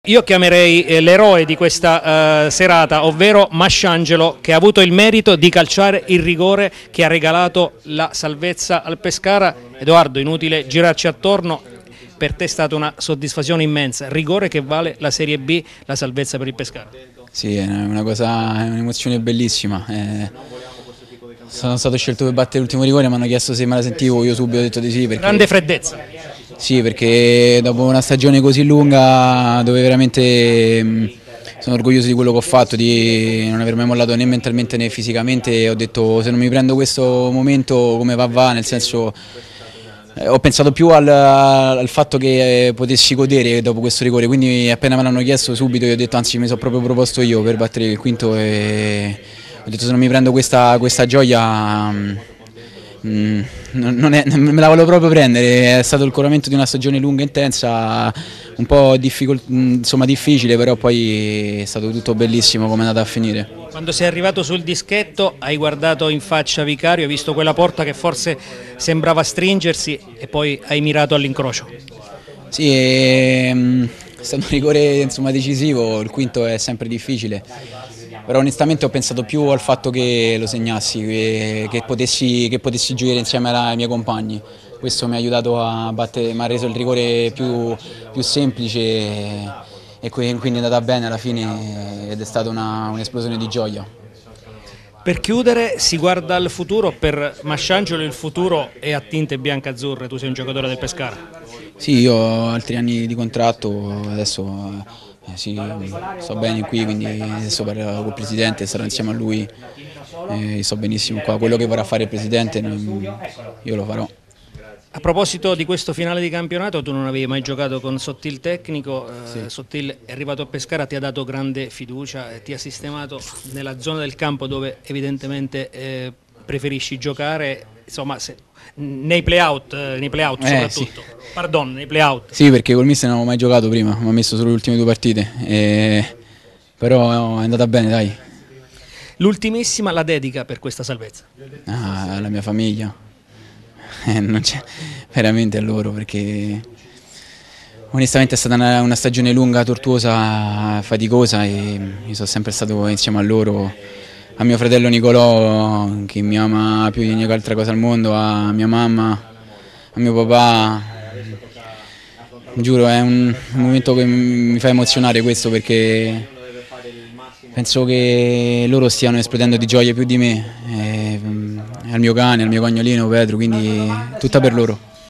Il Pescara resta in Serie B: Edoardo Masciangelo
Ai calci di rigore il Pescara supera il Perugia e resta in Serie B. Intervista post gara al difensore Edoardo Masciangelo che ha trasformato l'ultimo e decisivo rigore